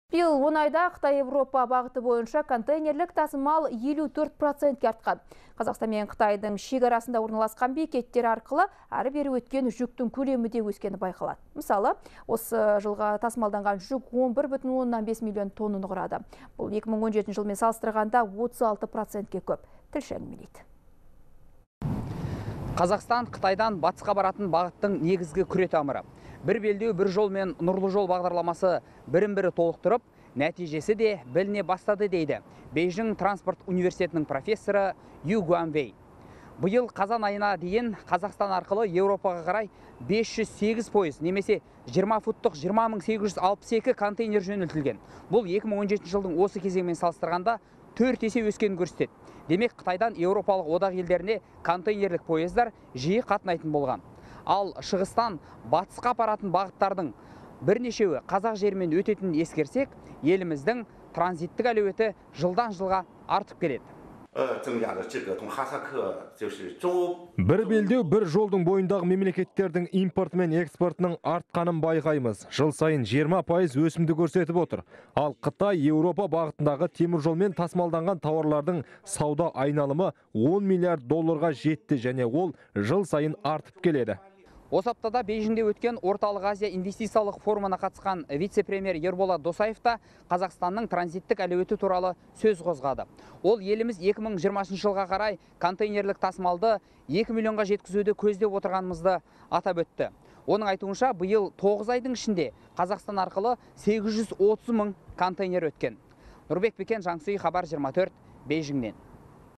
Қазақстан арқылы Қытай-Еуропа бағытындағы жүк тасымалы 54% артқан
Биыл 10 айда Қытай-Еуропа бағыты бойынша контейнерлік тасымал 54 процентке артқан, деп хабарлайды «Хабар 24».

Қазақстан мен Қытайдың шекарасында орналасқан бекеттер арқылы әрі-бері өткен жүктің көлемі де өскені байқалады. Мәселен, осы жылы тасымалданған жүк 11,5 миллион тоннаны құрады. Бұл 2017 жылмен салыстырғанда, 36 процентке көп.